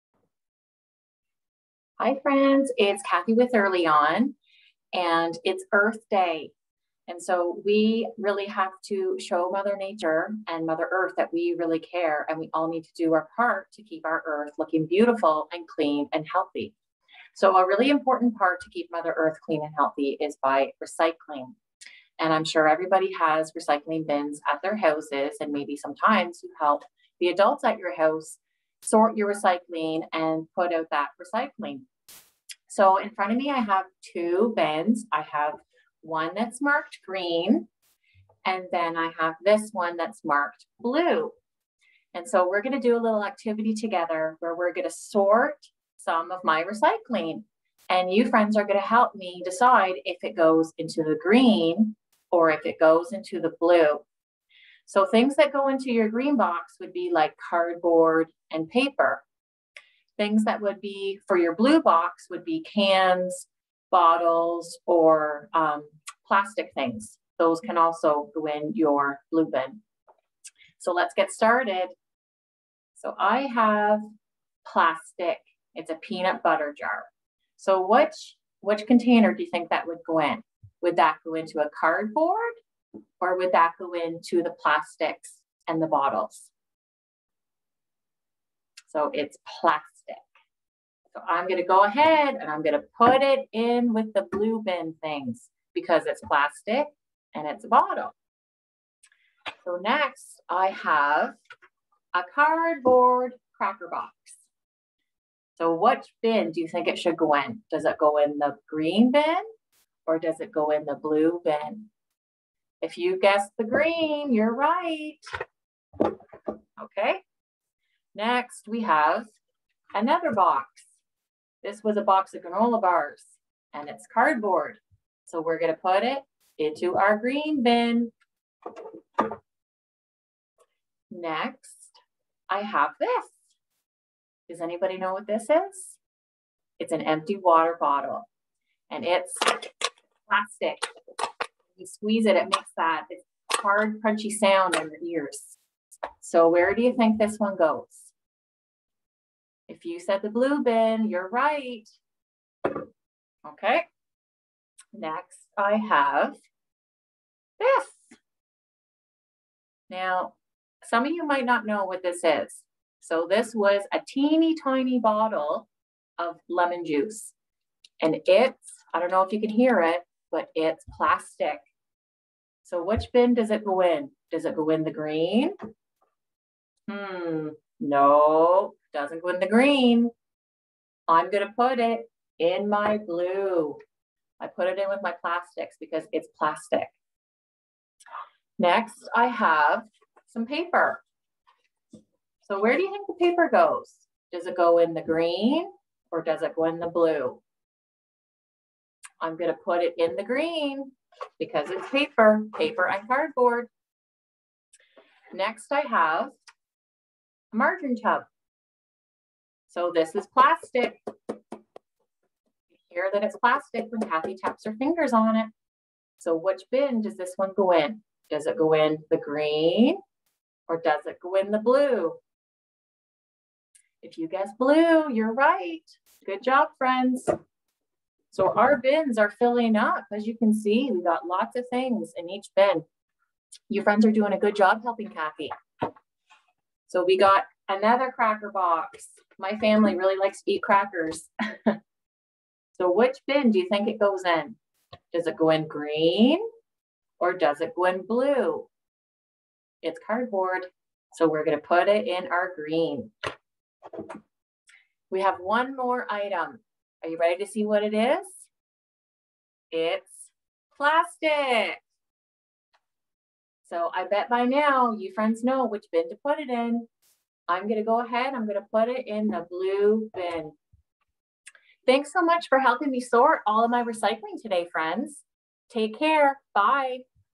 Sort some recycling
Help Cathy sort some recycling for Earth Day